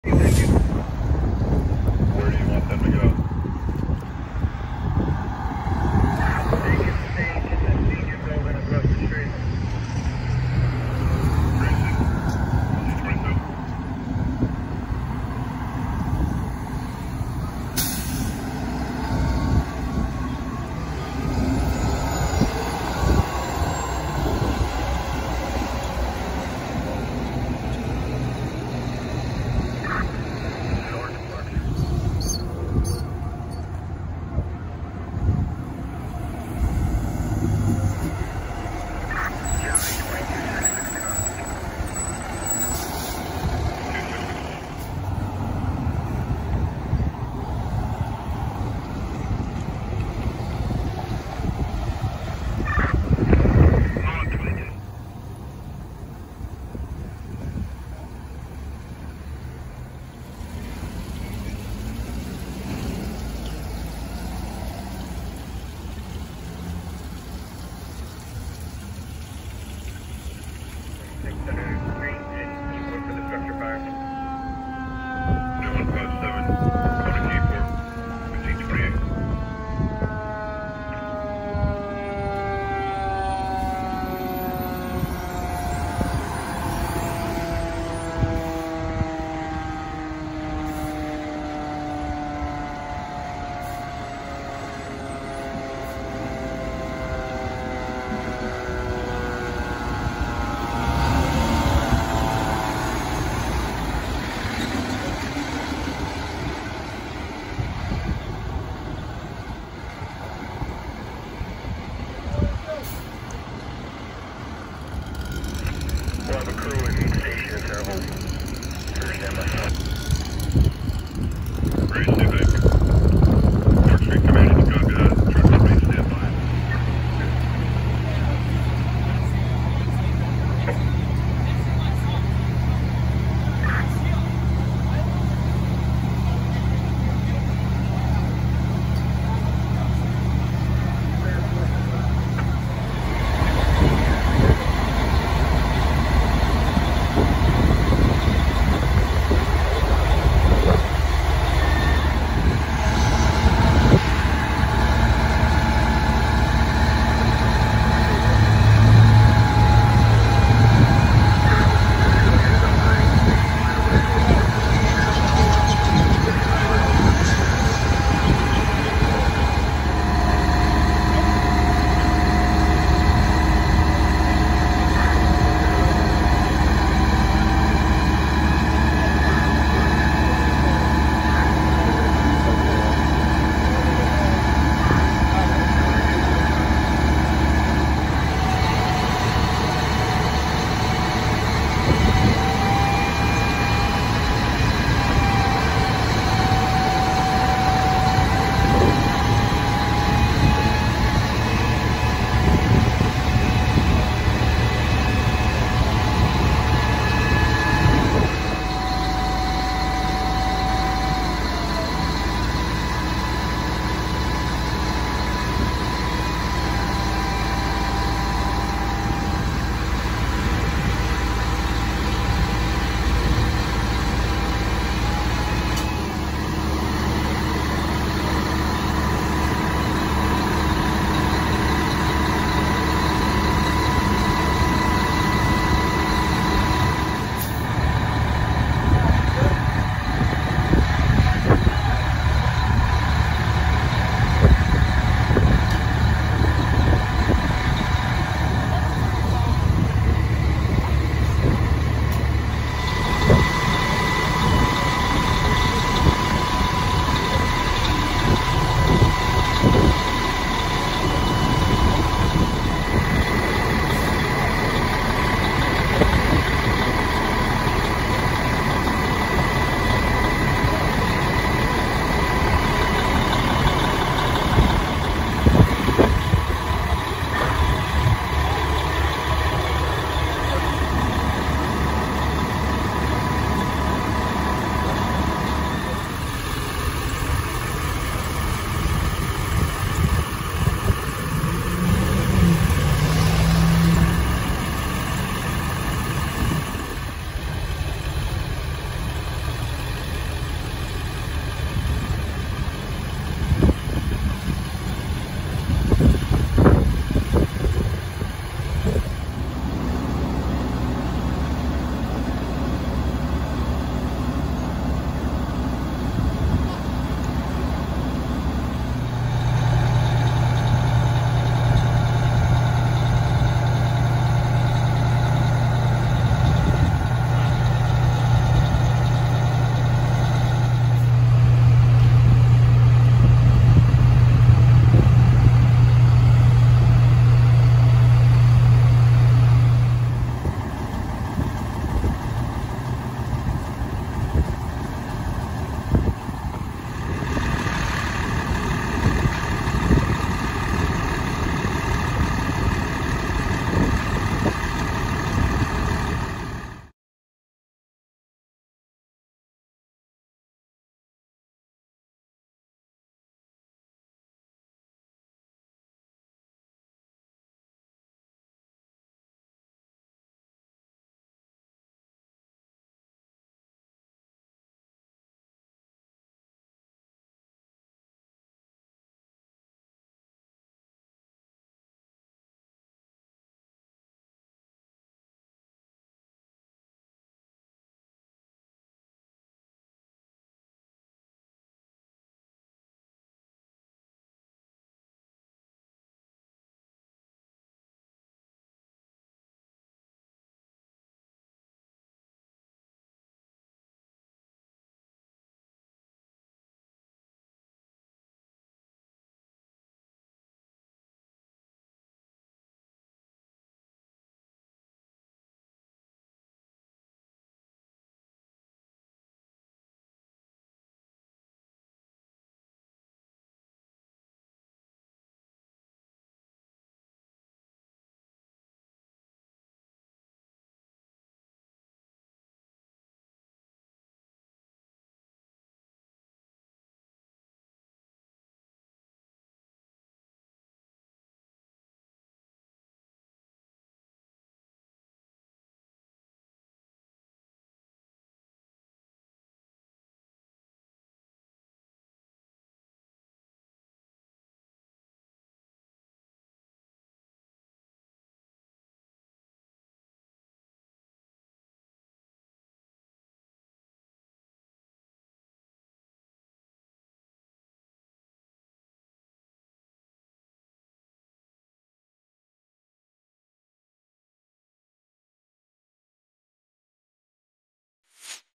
• Keyport NJ - 2nd Alarm Building Fire - Center St - 11/18/20 •
Location: Keyport NJ
Call Type: 2nd Alarm Structure Fire
Date Filmed: 11/18/20
Additional Info: N/A